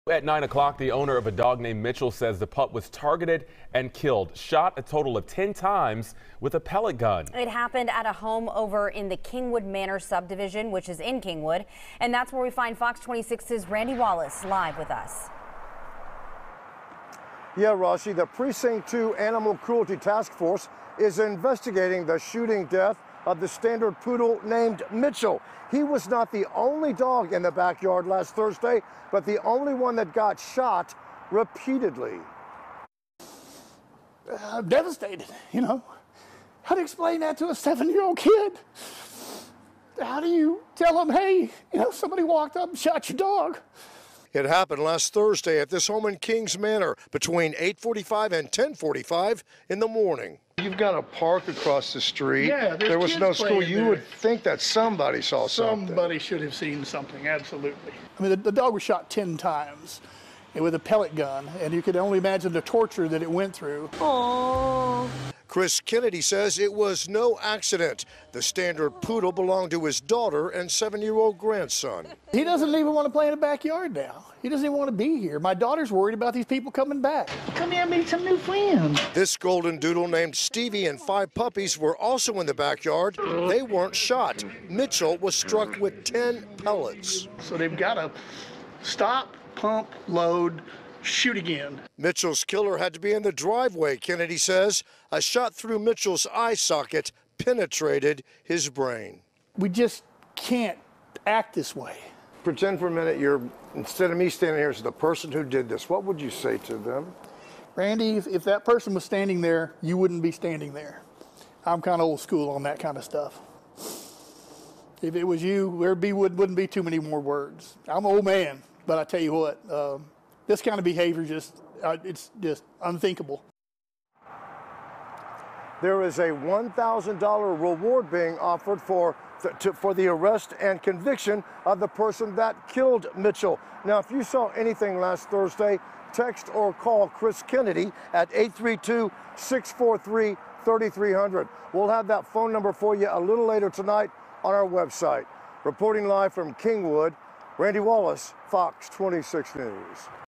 Dog shot multiple times by pellet gun dies
FOX 26 Reporter Randy Wallace spoke with the dog's owner, who is looking for the dog's killer.